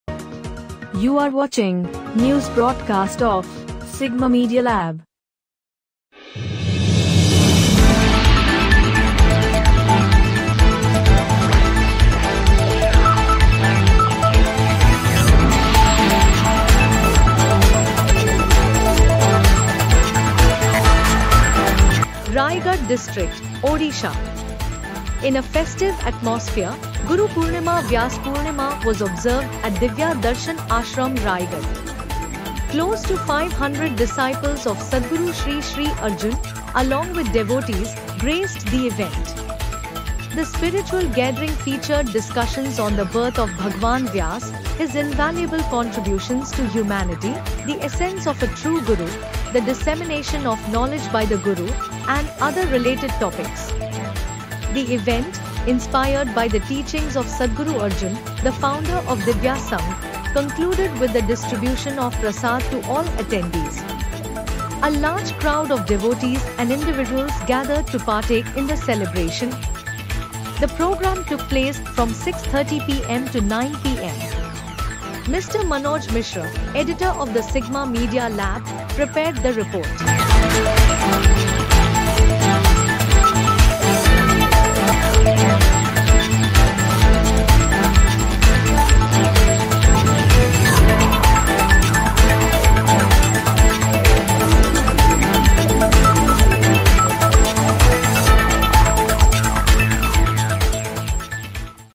Celebration of Guru Purnima (Vyas Purnima) at Divya Darshan Ashram, Rayagada.
Mr Manoj Mishra, Editor of the Sigma Media Lab, prepared the report.

 Rayagada District, Odisha. In a festive atmosphere, Guru Purnima / Vyas Purnima was observed at Divya Darshan Ashram Rayagada. A crowd of about five hundred people, including followers of Sadguru Sri Sri Arjun, attended the function.  The spiritual gathering featured discussions on the birth of Bhagwan Vyas, his invaluable contributions to humanity, the essence of a true Guru, the dissemination of knowledge by the Guru, and other related topics. The event, inspired by the teachings of Sadguru Arjun, the founder of Divya Sangh, concluded with the distribution of Prasad to all attendees. A large crowd of devotees and individuals gathered to partake in the celebration. The program took place from 6:30 p.m. to 9 p.m.